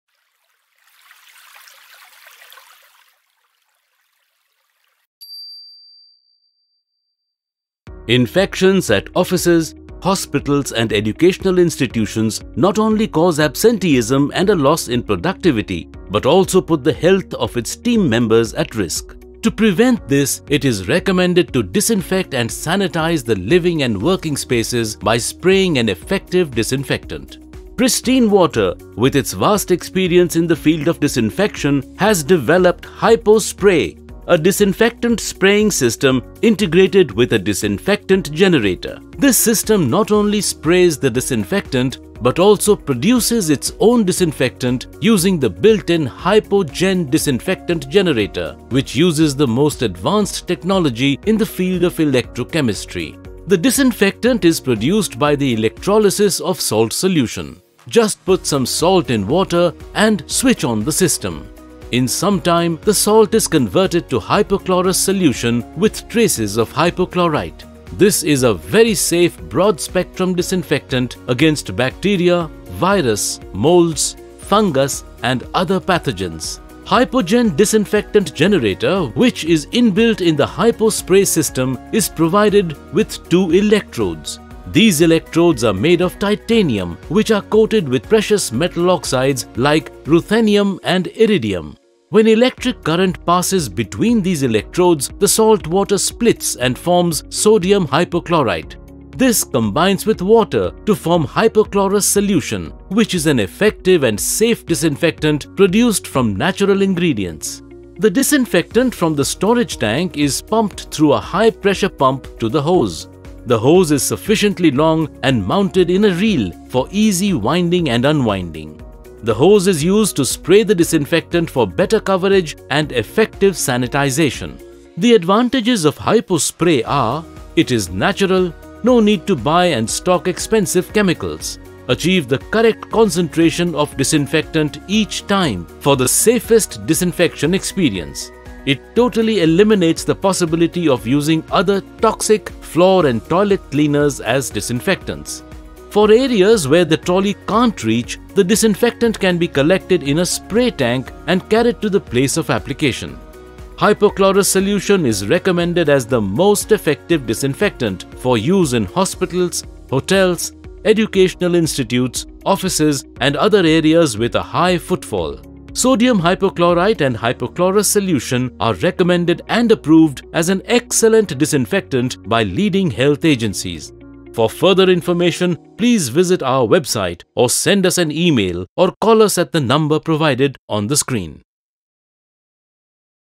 HypoSpray - Disinfectant Generation and Spraying Systems to disinfect Living and Working Spaces
Pristine Water presents HypoSpray® Disinfectant Spraying System with a built-in HypoGen Disinfectant Generator. To prevent the spread of infection due to human contact, during a pandemic, it has become very essential to disinfect and sanitize living and working space by spraying an effective disinfectant regularly.

Applications:
 - Disinfection of all Hospitals and Clinics to prevent HAI (Hospital Acquired Infections)
 - Sanitization of Schools, Colleges and other Educational institutions to protect students and staff from infections
Infection prevention in Commercial establishments like Offices, Factories, Malls, and Shops
 - Disinfection of Hotels, Restaurants and other food vendors
 - Sanitization of transport hubs like Airports, Bus Stands, Railway and Metro Stations
 - Arresting the spread of disease in areas of high footfall like Theatres, Places of worship, etc.
 - Disinfection of Housing complexes, Gated communities, and Residential colonies
 - Prevention of water-borne diseases through regular water tank disinfection
 - Disinfection of all water storage areas like Cooling Water, Fire Water and even Sewage tanks to prevent the growth of harmful pathogens